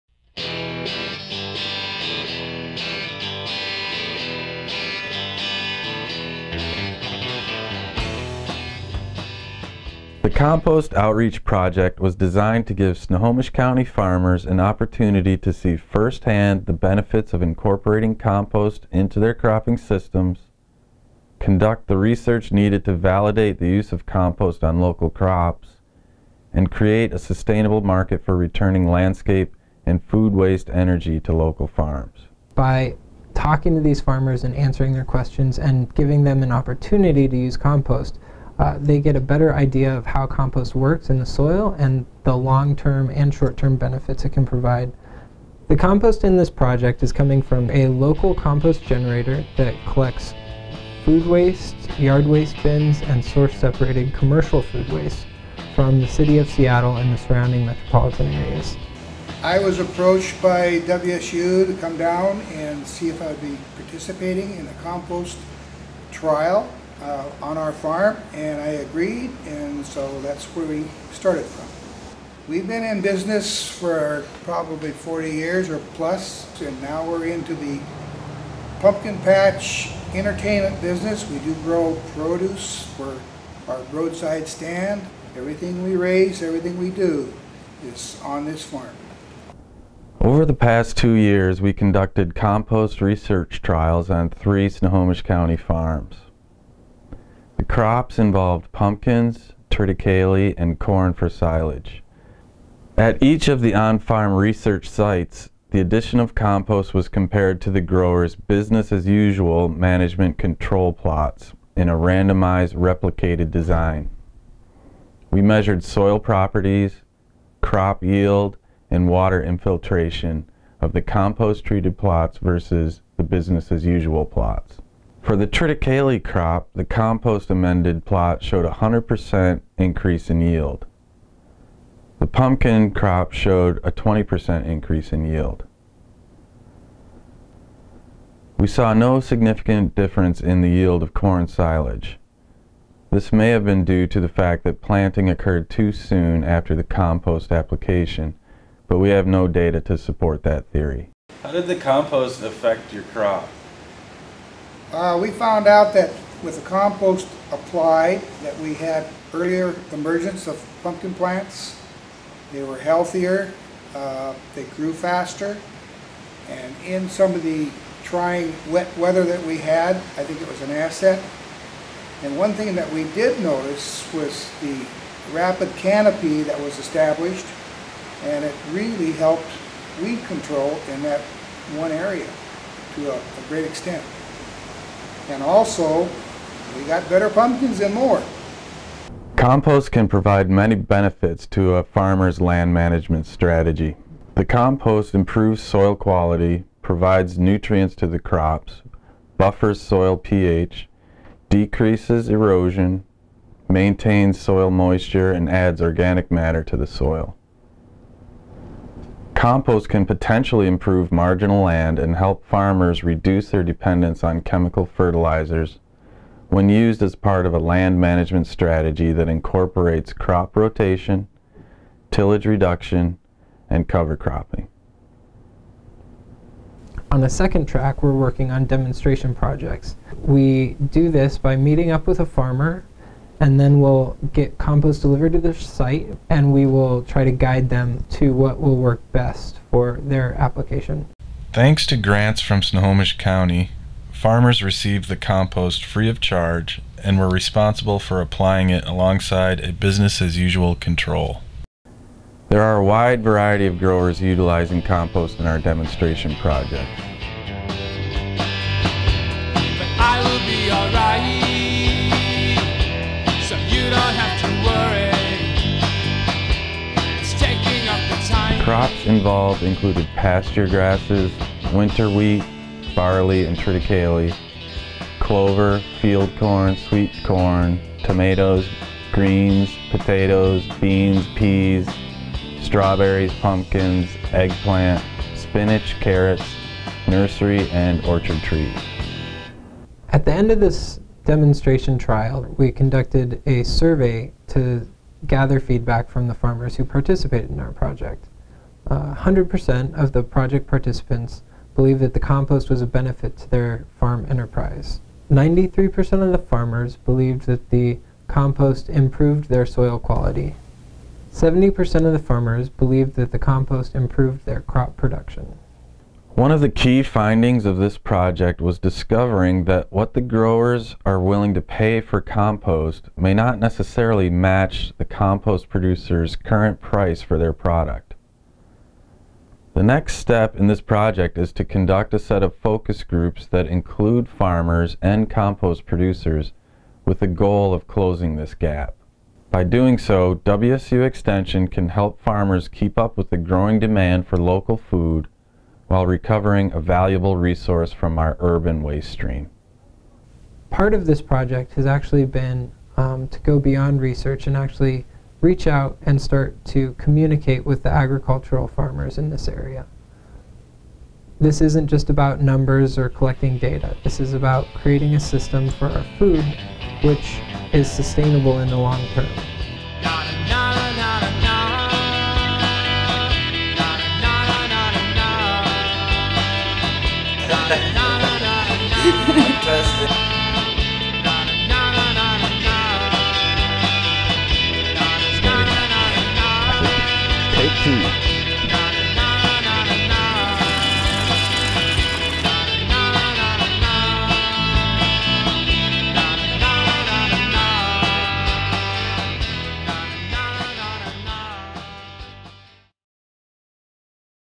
WSU Compost Outreach Project
Washington State University Snohomish County Extension Compost in Agriculture Outreach Project. In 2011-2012 on-farm compost research and demonstration trials were conducted in Snohomish County, WA. The goal of this project was to give farmers an opportunity to use compost on their crops, conduct the research needed to validate the use of compost in agriculture, and create a sustainable market for returning landscape and food waste energy to local farms.